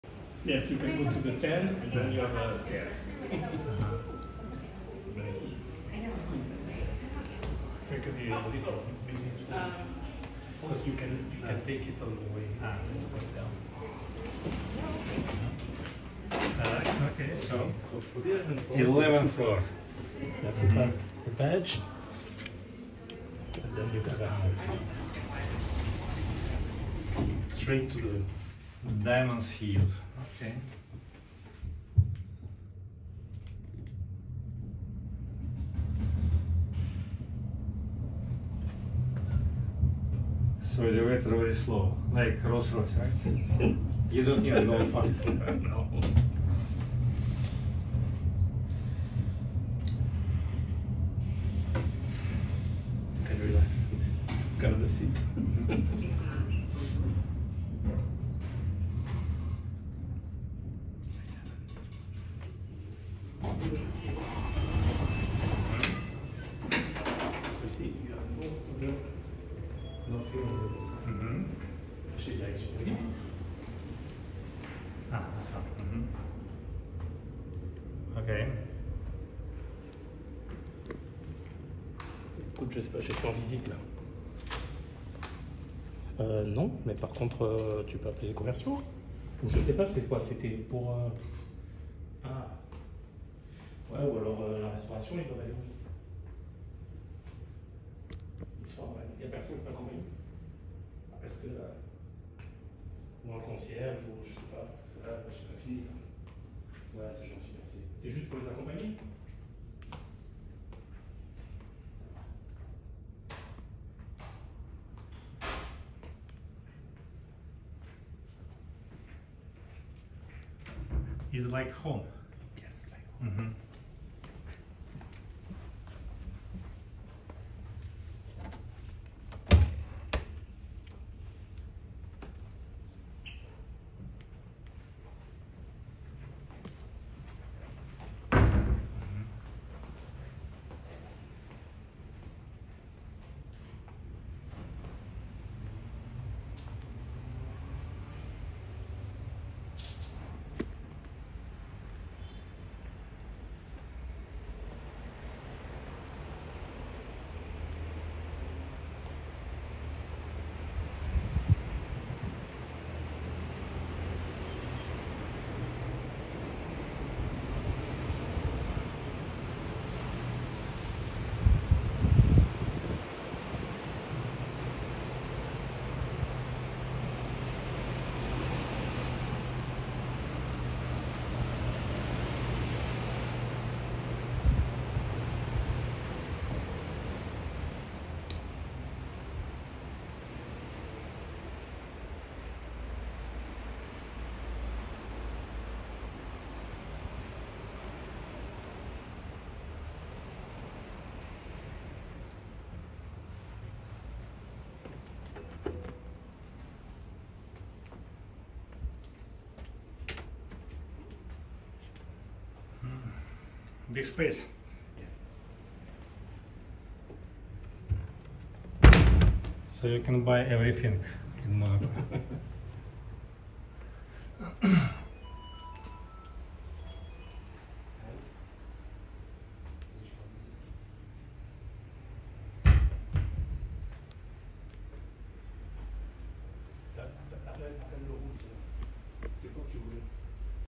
Monaco private residence | Monte-Carlo Bay Hotel & Resort | Diamond Suite Eleven – sea view | tour.
Start your day with an amazing view of the Mediterranean at the Monte-Carlo Bay Hotel & Resort’s Suite Eleven.

Imagine yourself up in the heavens looking down, or rather on the eleventh floor of this hotel. Take a private escalator and discover an exclusive suite offering top of the range amenities, with an elegant contemporary design in addition to innovative and easy-to-use technical features. Inside the suite, you’ll find a huge lounge-dining room and two bedrooms, in addition to a connection with a duplex suite. Featuring eucalyptus wood, natural stone, marble and mosaics, the suite combines natural materials with a sophisticated design in a delicate balance creating a warm and intimate atmosphere. Out on the terrace, discover a 180° view across the Mediterranean.